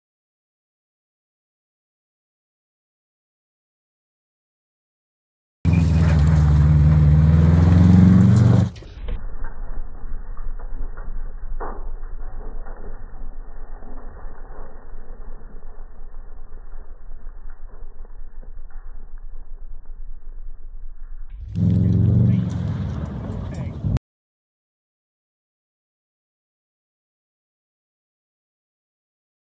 Slowmotion Booty Shakin -- KWRC RallyCross #2
Matt Tregunno teaching his girl how to dance at KWRC's Rallycross